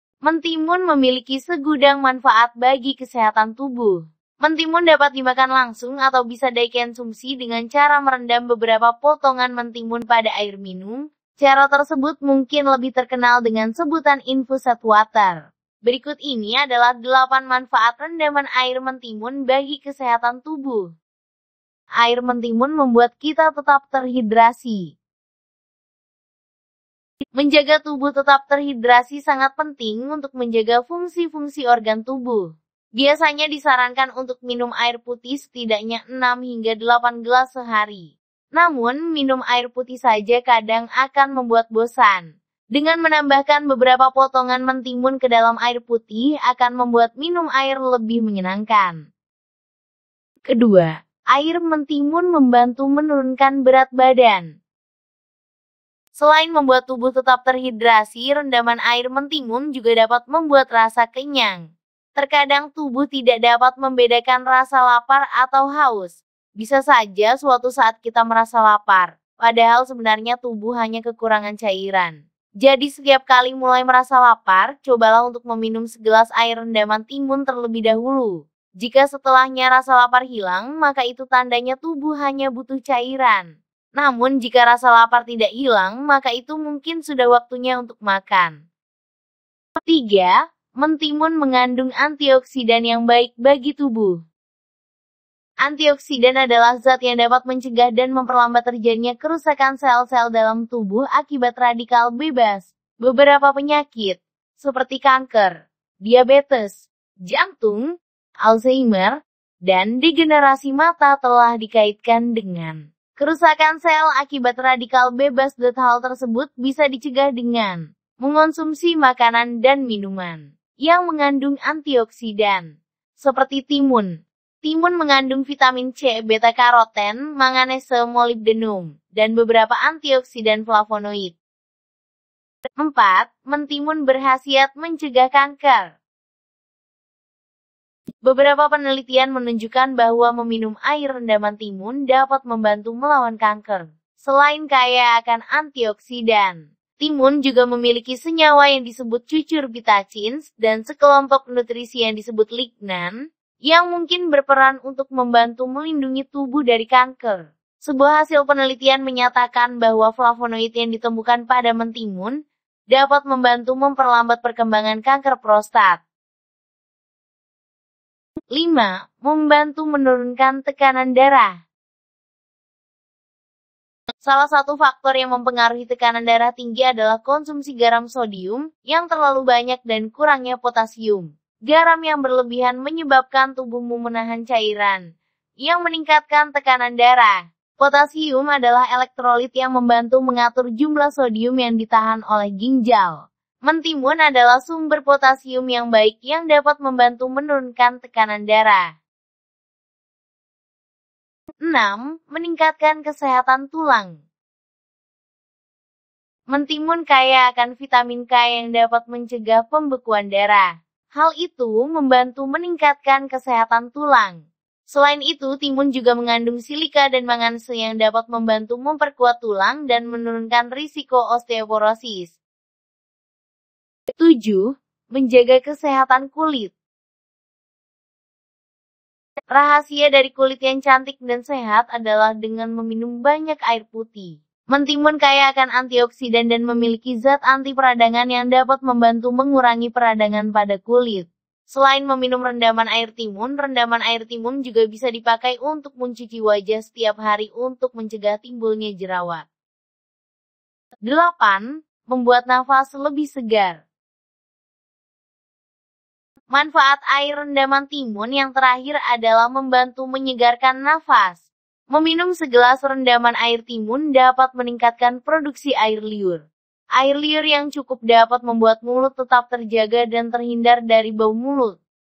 8 Manfaat Infused Water (Rendaman Mentimun) Bagi Kesehatan, Diantaranya Menurunkan Tekanan Darah.
Mentimun bisa dimakan langsung atau dibuat infused water, atau air rendaman mentimun.Mentimun kaya dengan air, vitamin, mineral dan beberapa zat yang sangat baik untuk kesehatan. Infused water mentimun memiliki manfaat antara lain :
merehidrasi tubuh,
mengurangi rasa lapar,
menurunkan berat badan,
sumber antioksidan,
mencegah kanker,
kesehatan tulang,
kesehatan kulit,
menyegarkan nafas,